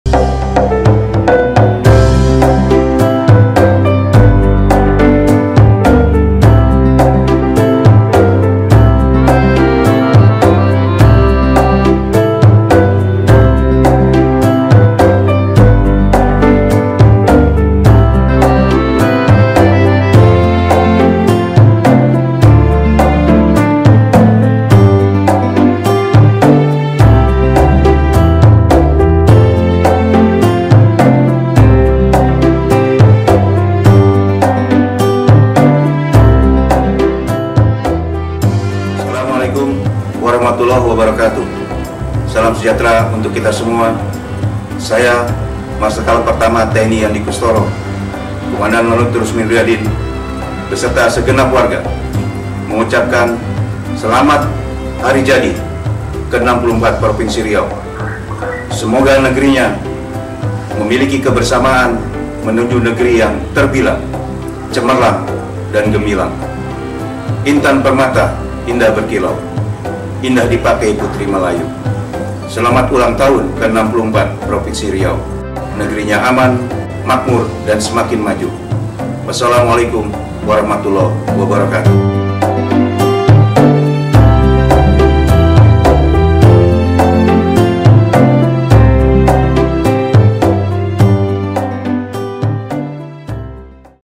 Selamat Hari Jadi Ke-64 Provinsi Riau dari Lanud Roesmin Nurjadin
Komandan dan Ketua PIA Ardhya Garini Cabang 12/D.I beserta Keluarga Besar Lanud Roesmin Nurjadin, Mengucapkan
Selamat Hari Jadi Ke-64 Provinsi Riau
9 Agustus 1957 - 9 Agustus 2021